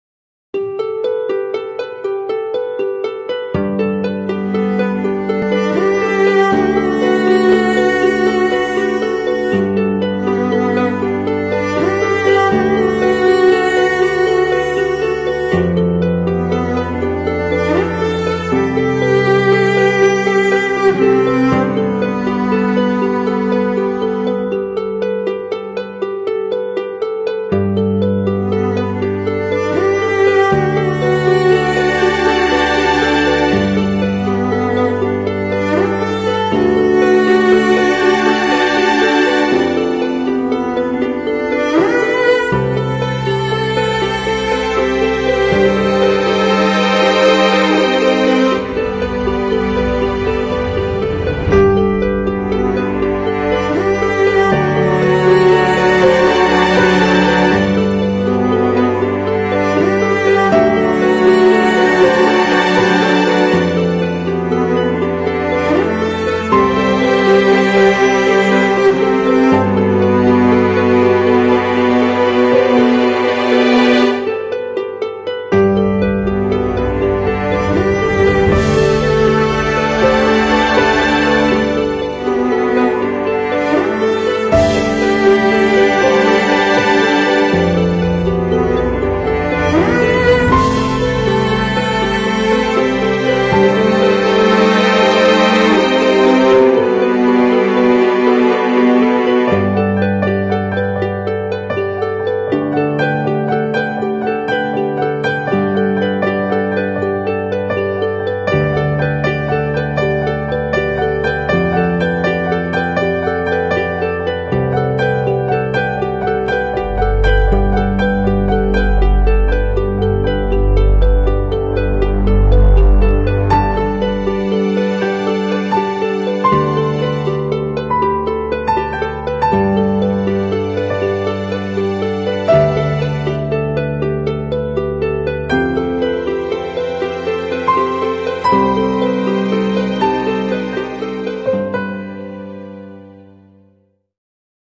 Emotional Poem/Story Background Music
This background music is very sad and emotional music. You can use this music, poem/poetry, recitation, sad story, drama, emotional dialogue, cinematic movie, video, YouTube video and etc.

🙏Thanks for listening the music🎶